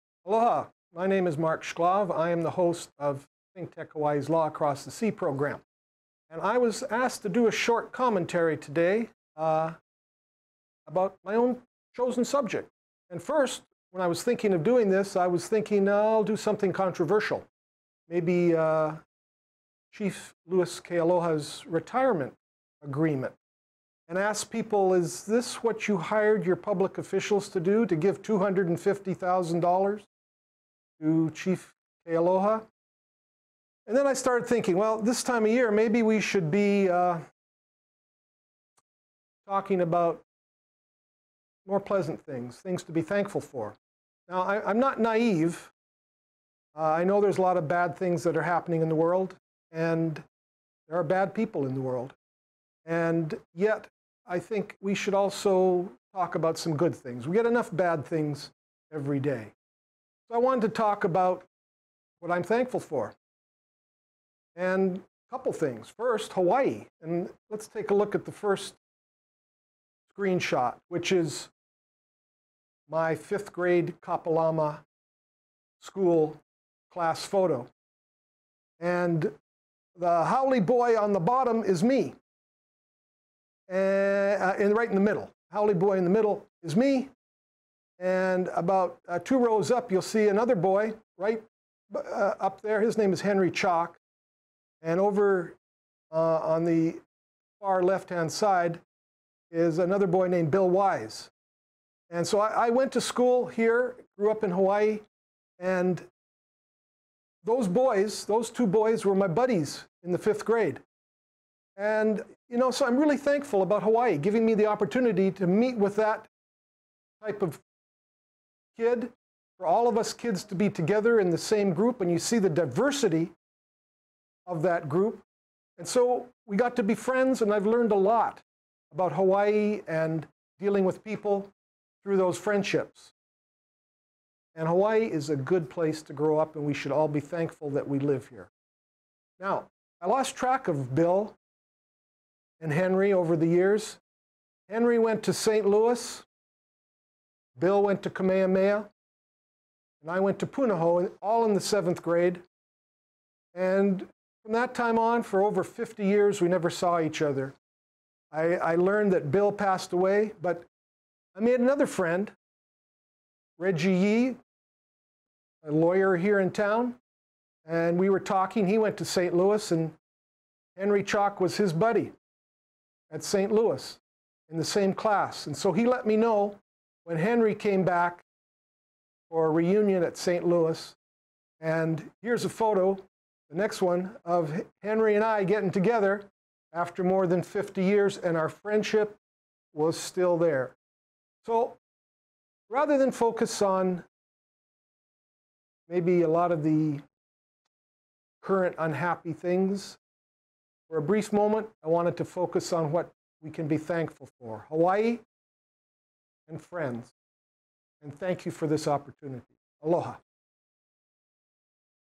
Being Thankful (ThinkTech Commentary)
Mark Shklov reflects on why it's important to be thankful this holiday season.